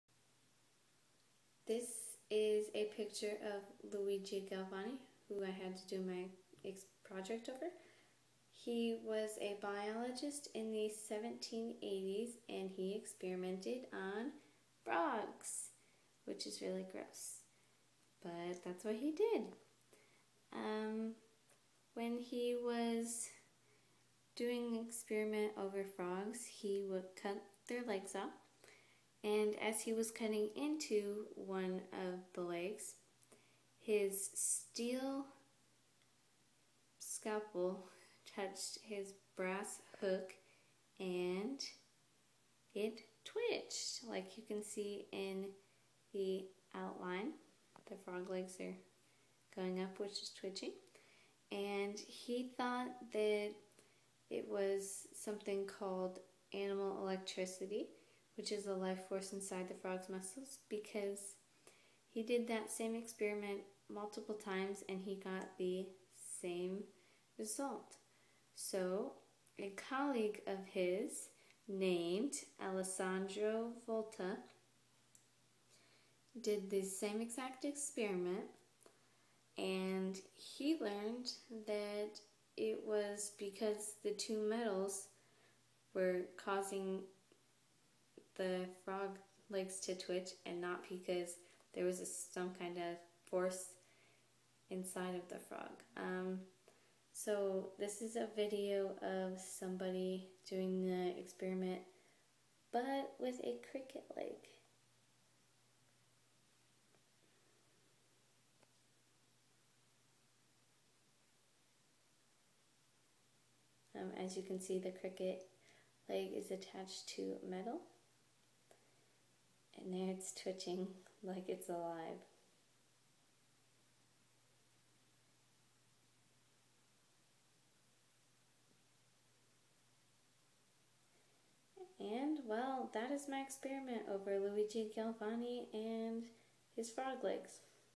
Luigi Galvani and experimenting on frogs
Luigi Galvani experimented on a frog's leg and discovered something new.